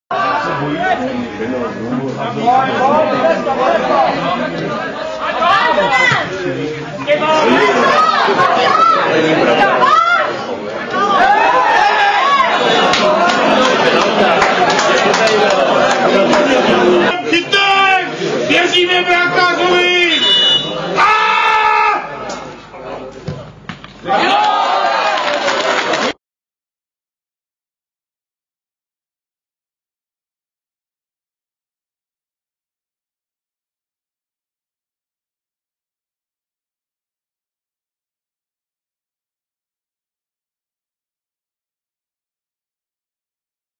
Šluknovský fotbal 2009/10 : Šluknov - Libouchec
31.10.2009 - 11.kolo I.A třídy - obrovská šance našeho soupeře na zvýšení skóre